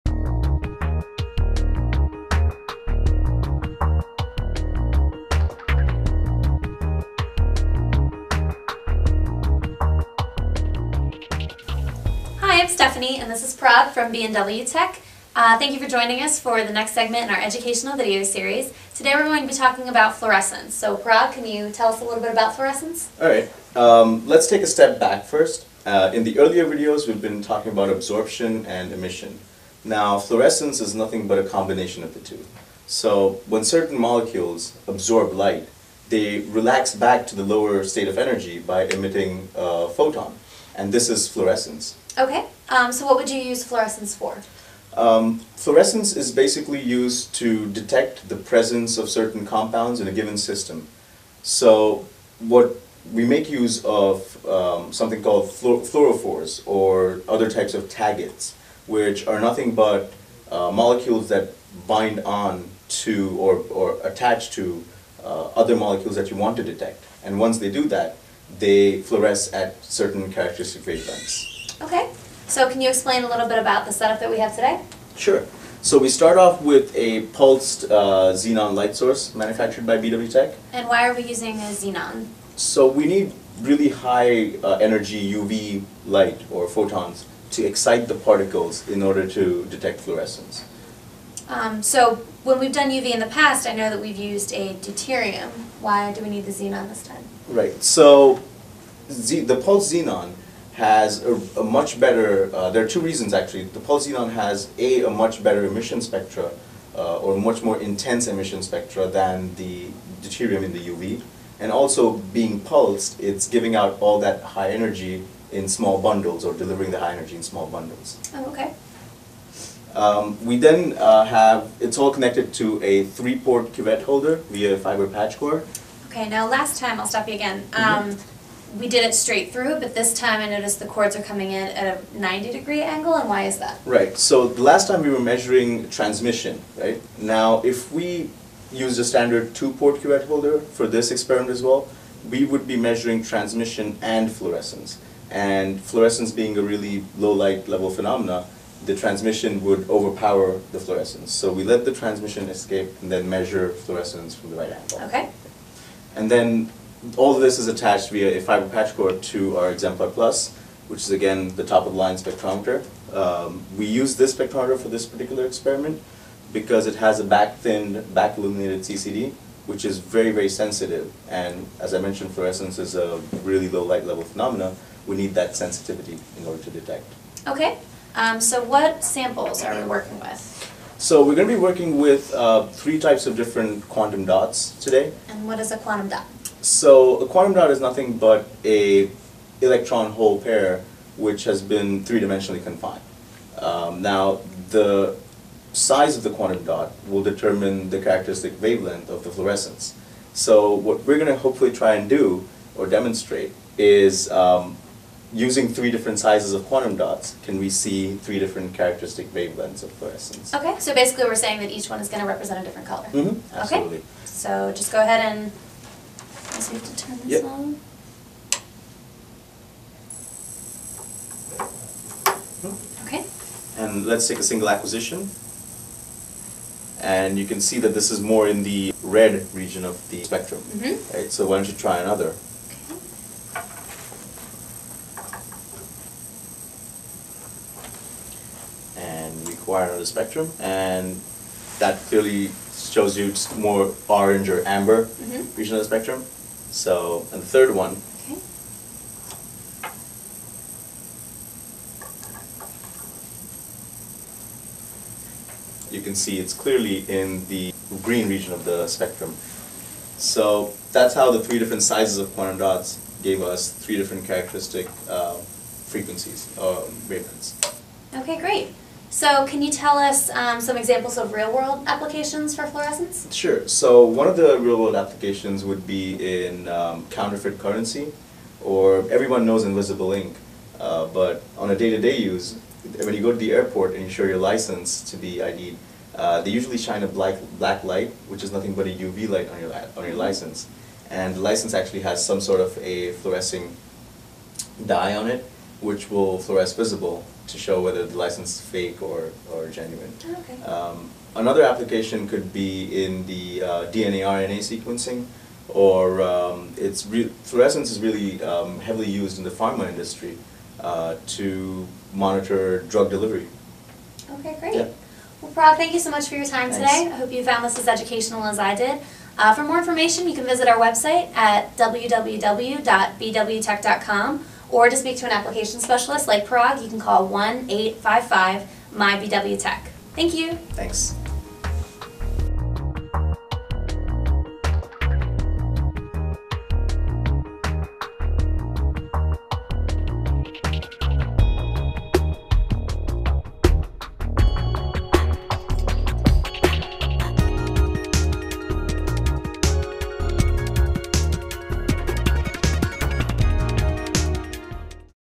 Educational Series: What is Fluorescence Spectroscopy?
In this episode of B&W Tek's Educational Video Series we discuss fluorescence.  Our discussion will include an overview of some basic principles, a demonstration of taking measurements, and some common applications.